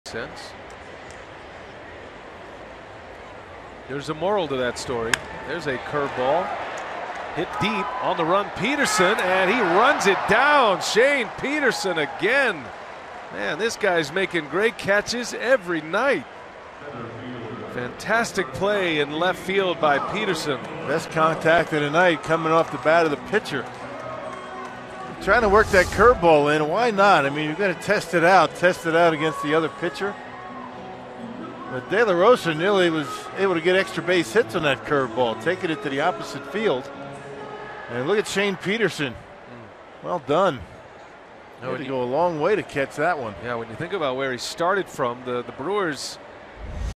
MIL@COL: Peterson makes a nice running catch in left
6/19/15: Shane Peterson makes a nice catch on the run in left, taking away an extra-base hit from the Rockies